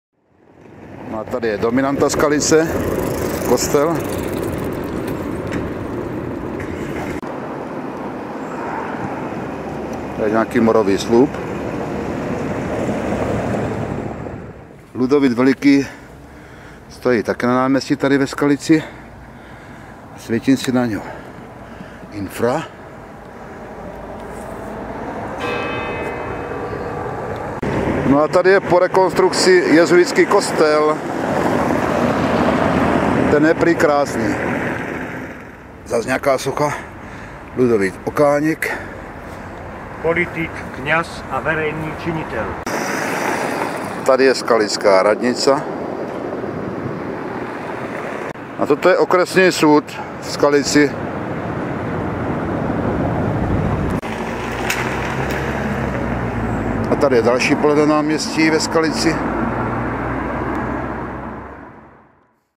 PRÁVĚ DNES - SKALICA (SK) - a její památky v pár sekundách v centru města
- história prihraničného města Skalica je skutečně veliká a je tam co vidět:-):-):-)